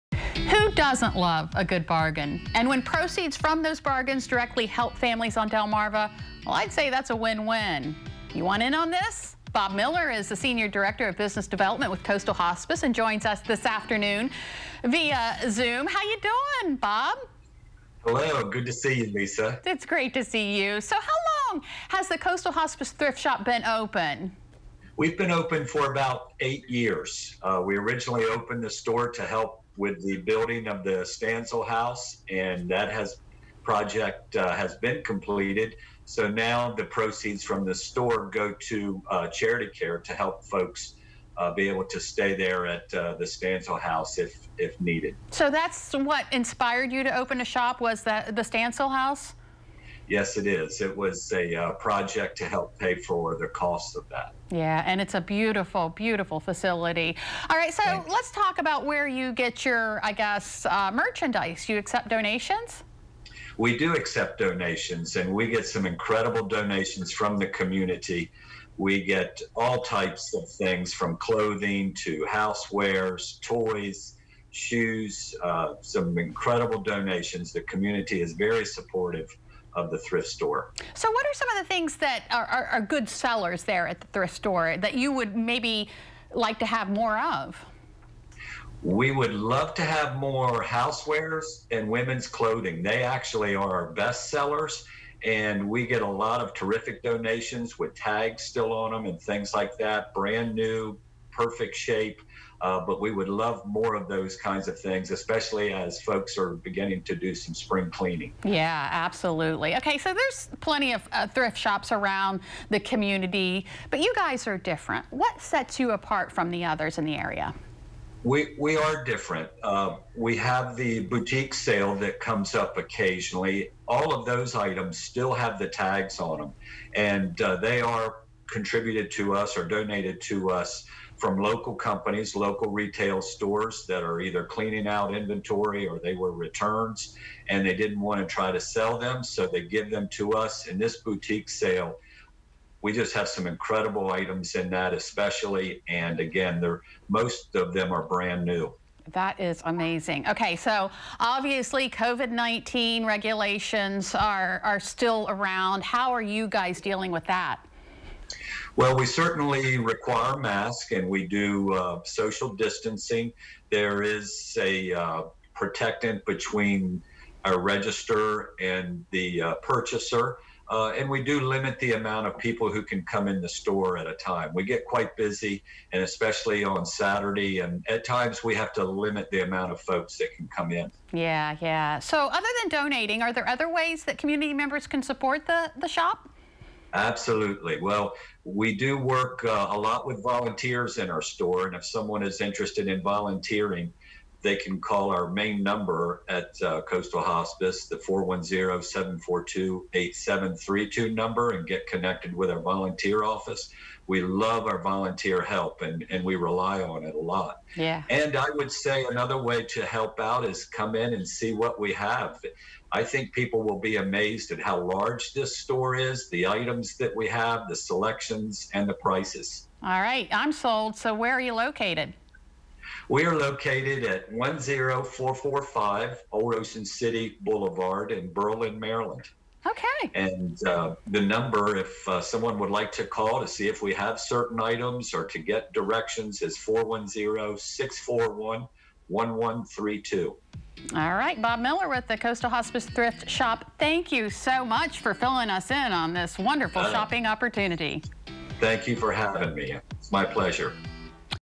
Coastal Hospice Thrift Shop - Berlin
There's a place to find good bargain's in Berlin. Coastal Hospice opened a thrift shop a few years ago and, as we find out, it provides a different way to shop.